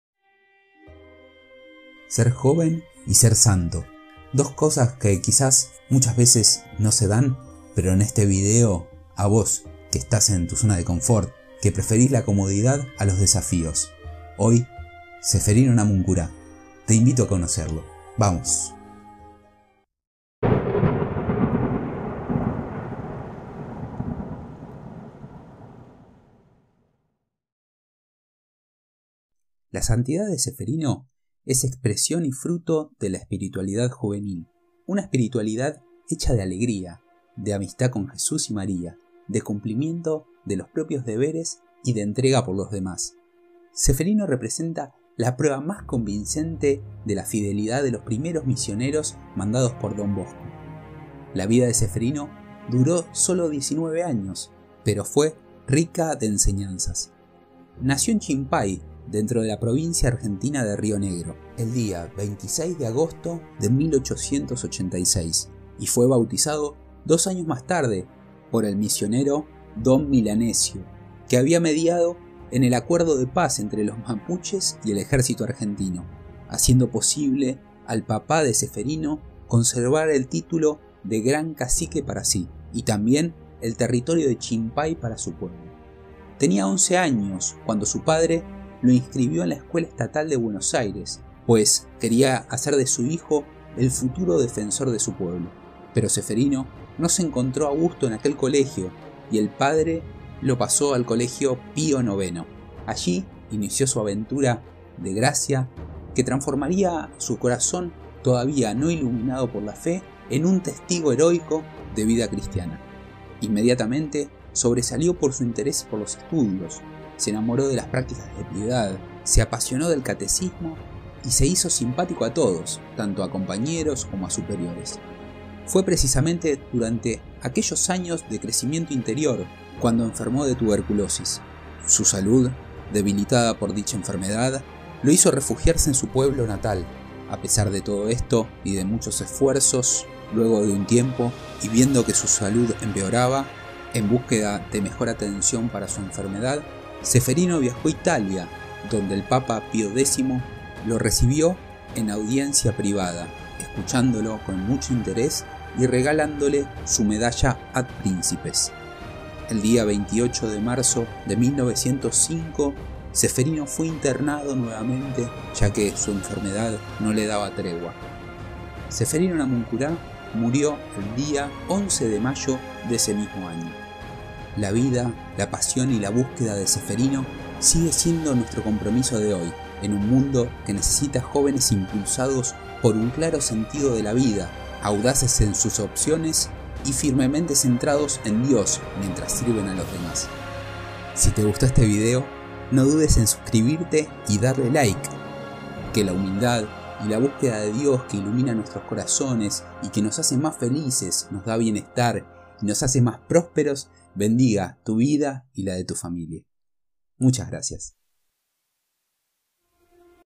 CEFERINO NAMUNCURA -Conoce su Vida en ⏰ 3 minutos.
Conoce en este video un poco más de la vida de Ceferino Namuncurá. Compartí  un recorrido único con un mensaje donde la fe es un camino de inspiración, mejora, cambio y plenitud.

⭕ Si querés ver otro de nuestros videos